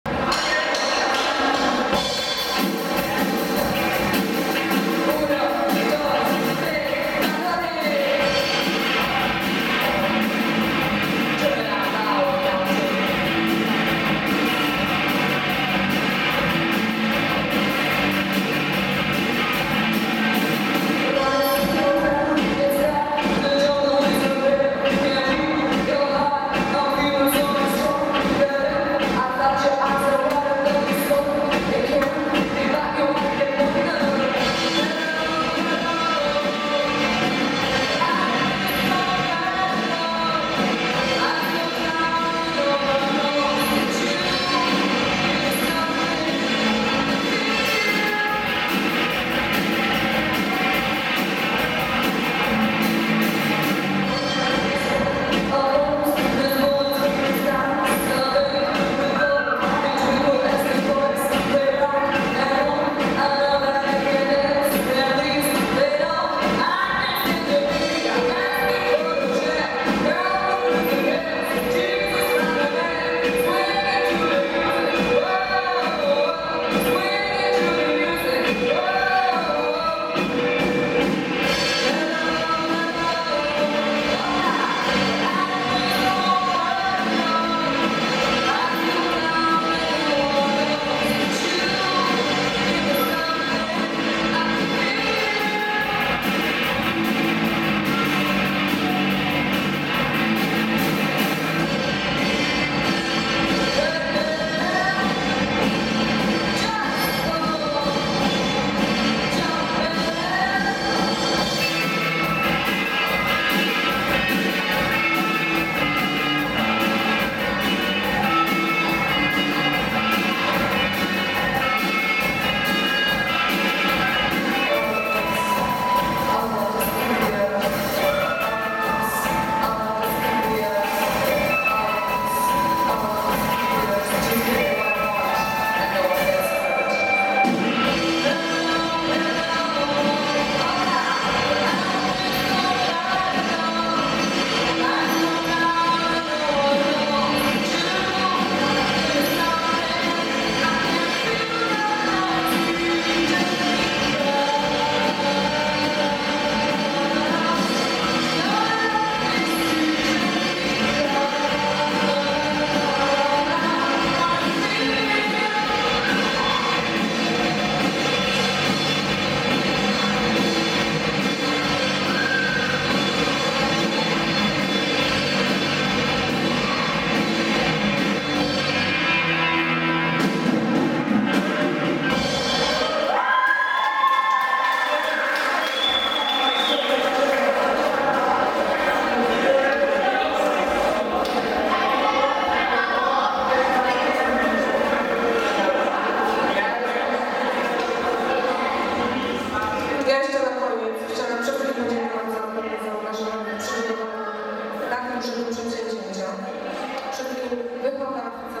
u2 - sara, ula, jasiek, michal
Brzostek, Zyzek, Sieczka, Baranowska - zespół I'm with You  jako zdobywca nagrody publiczności  w ramach organizowanego przez szkoły katolickie w Szczecinie Mam Talent gra piosenkę U2.